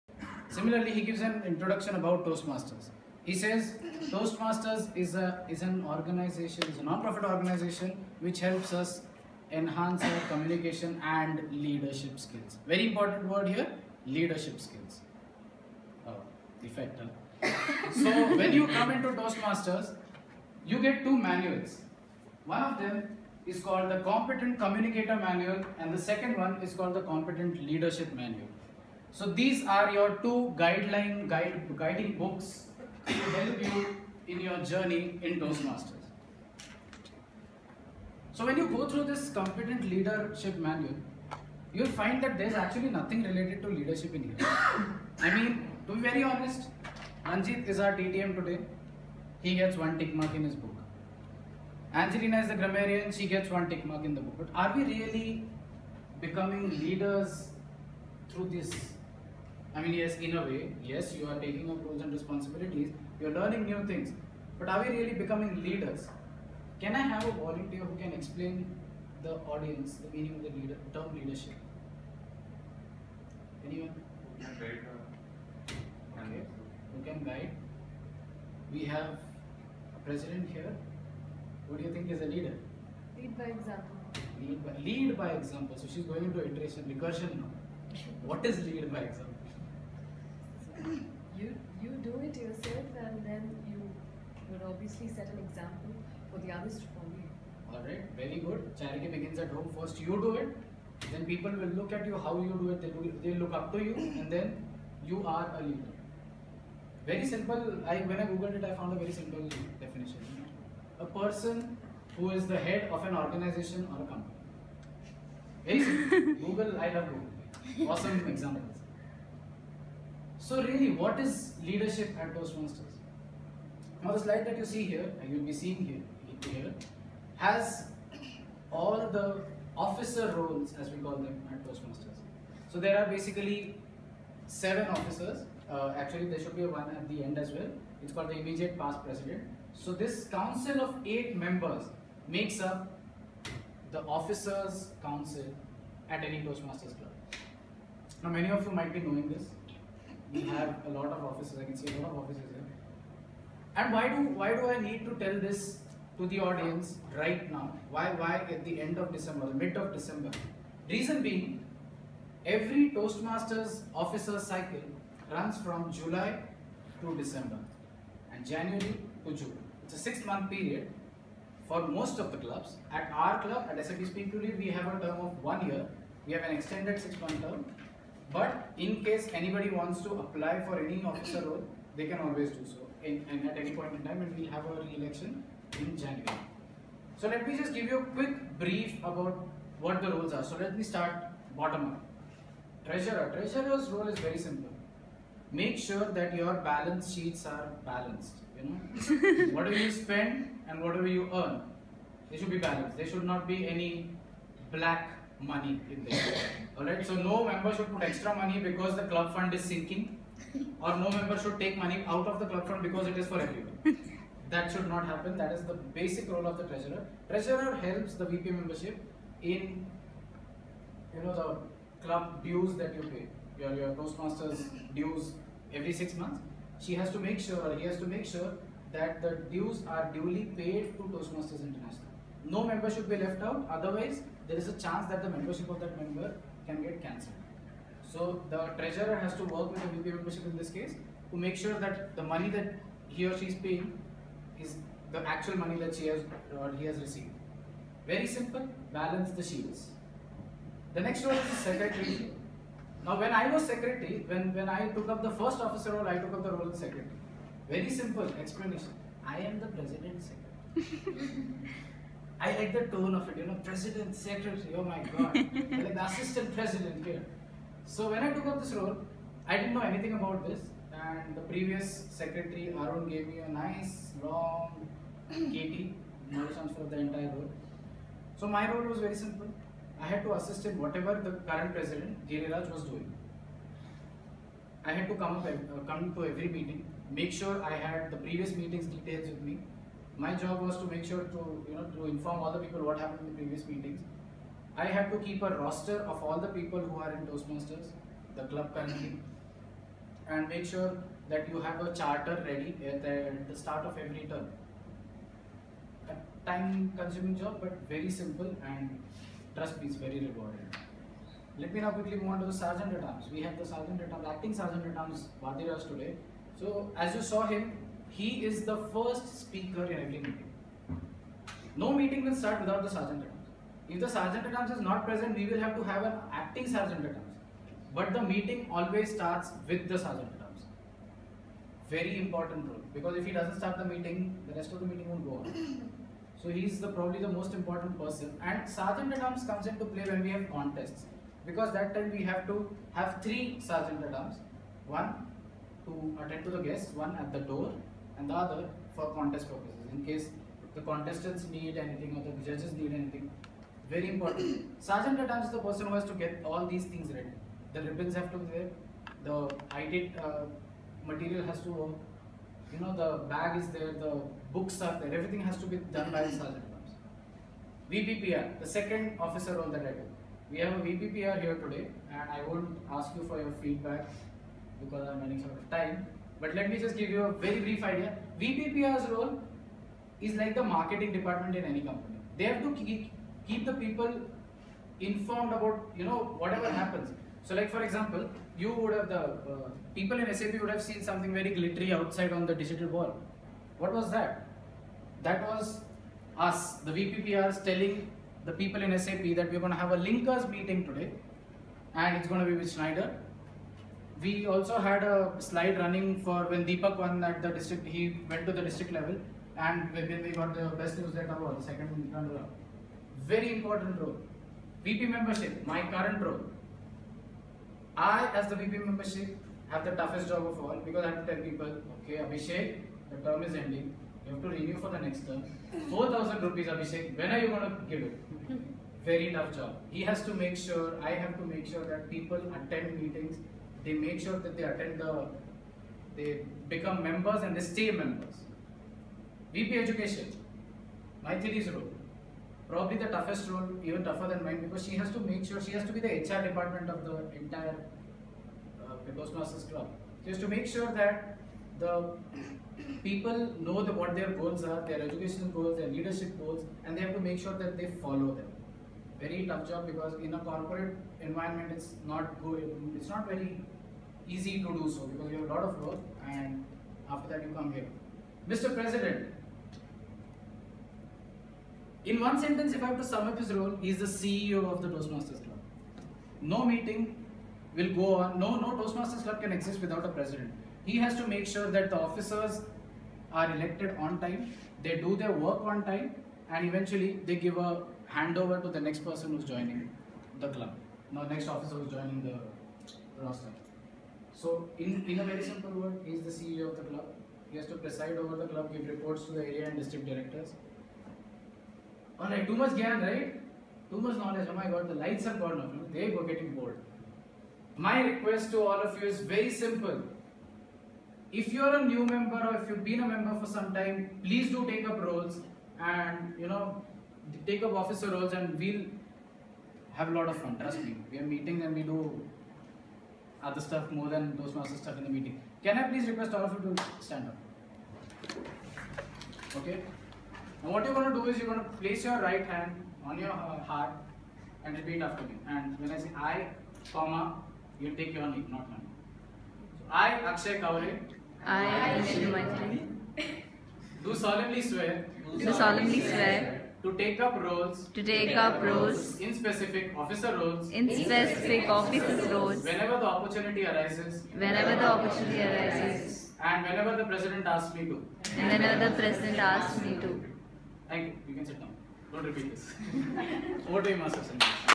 Toastmaster Akshay giving his CC10 speech
TM Akshay Giving his CC10 on the Topic : Leadership@Toastmasters